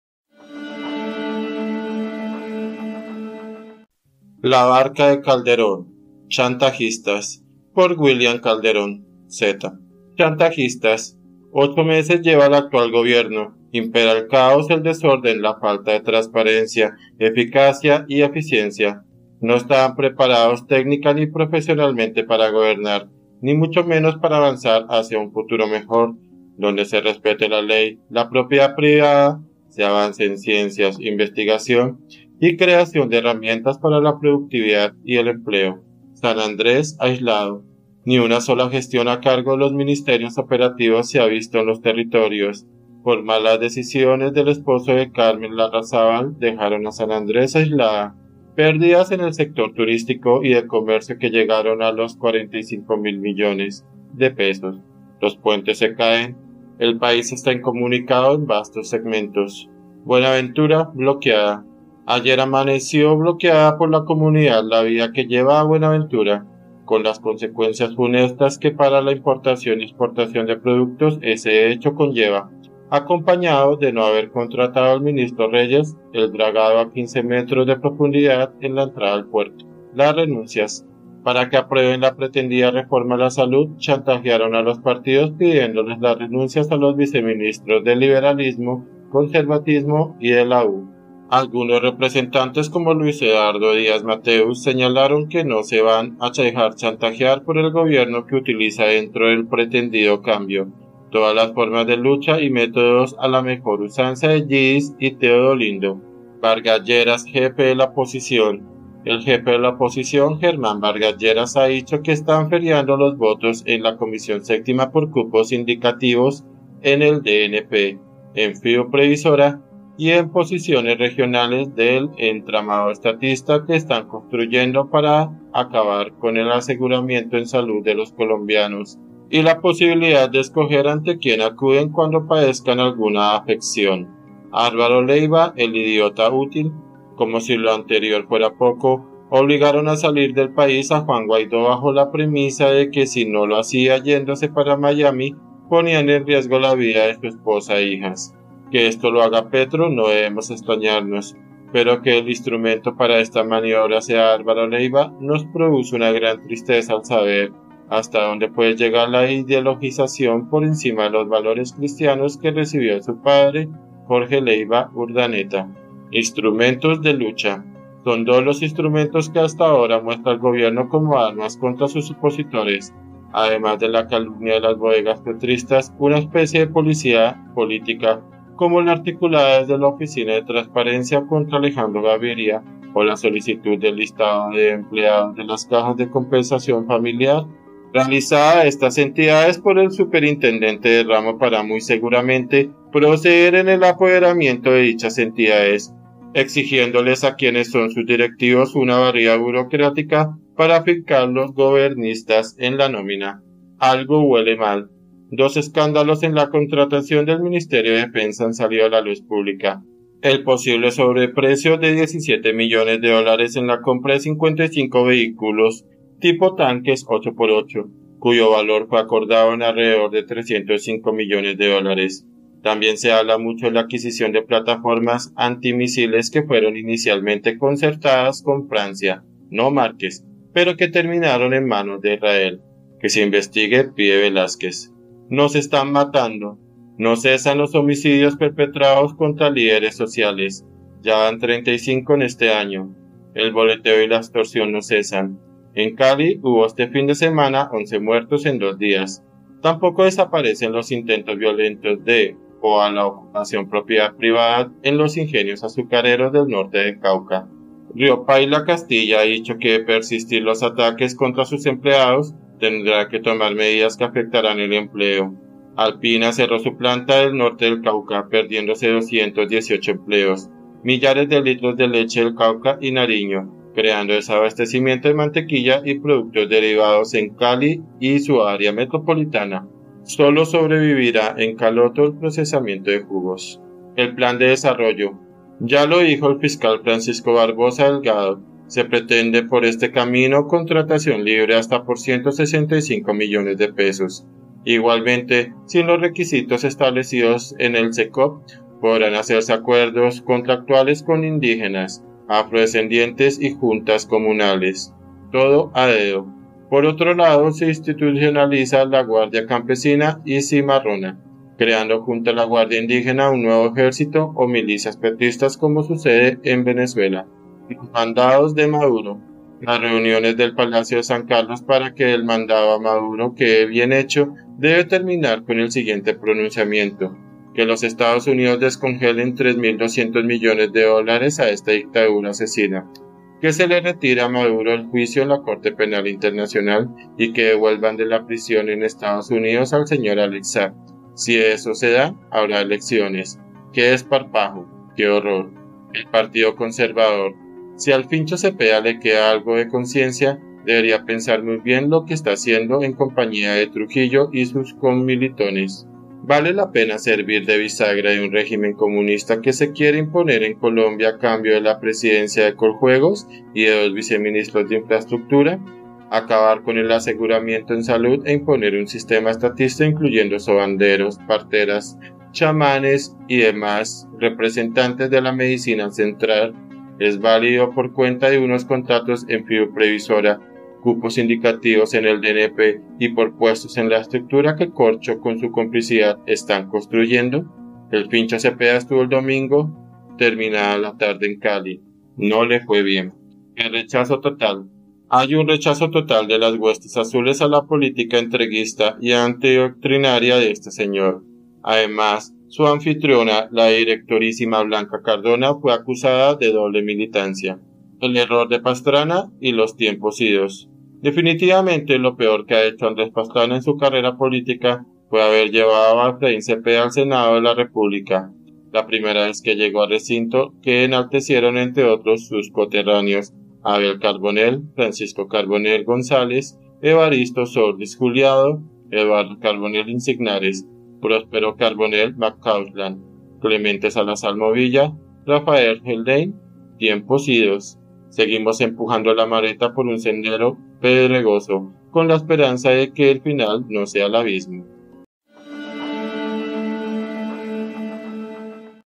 CHANTAJISTAS 🔴 Por William Calderón Z
Ocho meses lleva el actual gobierno. Impera el caos, el desorden, la falta de transparencia, eficacia y eficiencia. No estaban preparados técnica ni profesionalmente para gobernar, ni mucho menos para avanzar hacia un futuro mejor, donde se respete la ley, la propiedad privada, se avance en ciencias, investigación y creación de herramientas para la productividad y el empleo.
Por William Calderón Z